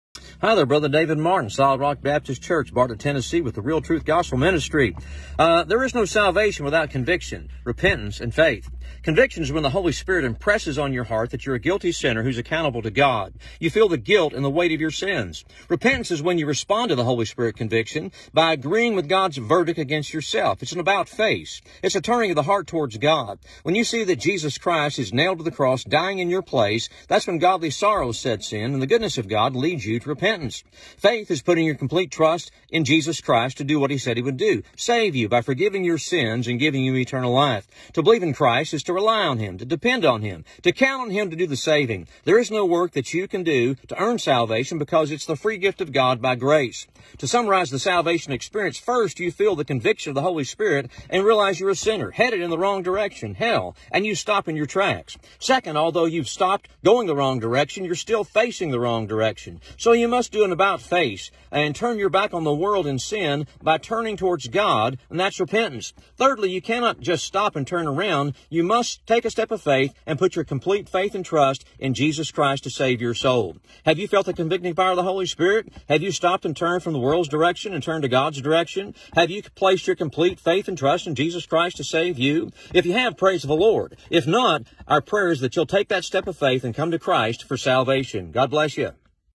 REEL TRUTH: "Have You Been Convicted?"
If you have been CONVICTED, these are some things you NEED TO KNOW.

IF YOU ARE NOT a CHRISTIAN here’s how you can become one